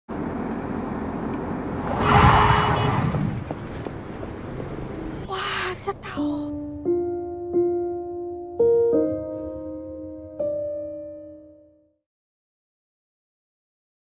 ทำไมผู้หญิงคนนี้ถึงวิ่งกลางถนน ... แปลกนะ !
แปลก อยู่ดี ๆ ผู้หญิงคนนี้มาวิ่งกลางถนนทำไม?
ติดตามเรื่องราวของเธอได้เร็ว ๆ นี้!